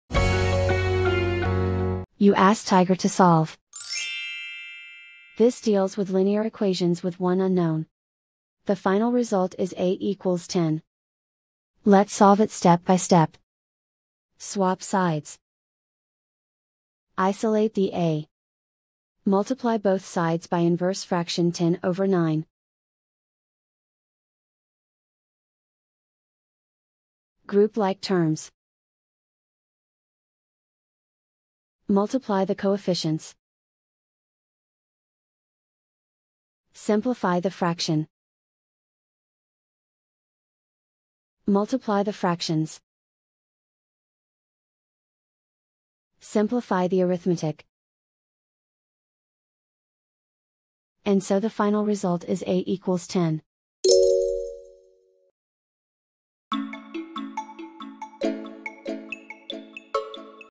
Solve 9=9/10a: Linear Equation Video Solution | Tiger Algebra
Struggling with 9=9/10a? Watch our step-by-step video on Tiger Algebra to master this linear equation effortlessly.